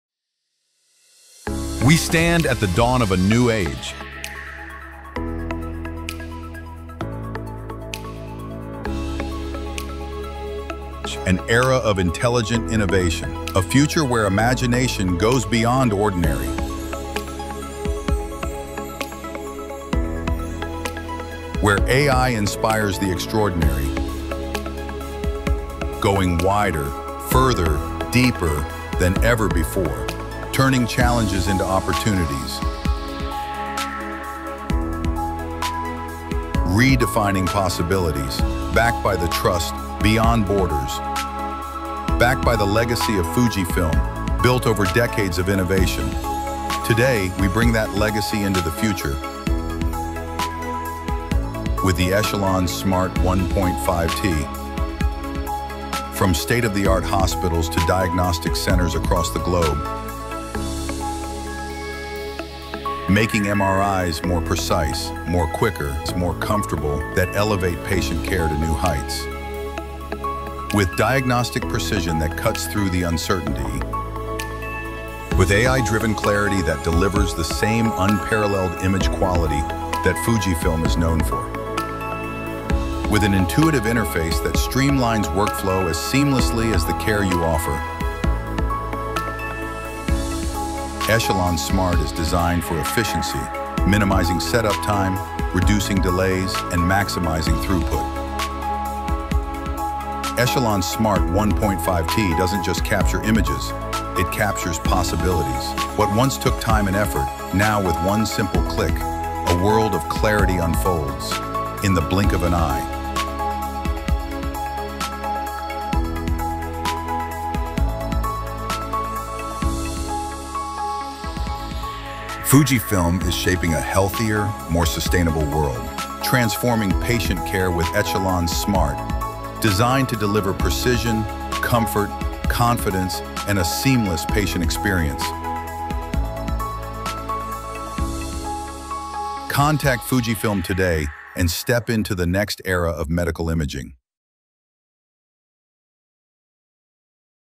Comfort, Clarity & Efficiency | Discover ECHELON Smart MRI
Smaller footprint. Bigger possibilities.
Fujifilm ECHELON Smart MRI system is designed to redefine the way we think about superconductive MRI. Cost-efficient, space-saving, and patient-first, without compromising diagnostic excellence.

Powered by Fujifilm’s proprietary technologies, ECHELON Smart delivers:
✔ Quieter exams with up to 96% noise reduction
✔ Faster workflows and reduced scan times
✔ Superior image quality for accurate diagnostics
✔ Eco-friendly operations with reduced running costs

Because smarter imaging means smarter healthcare.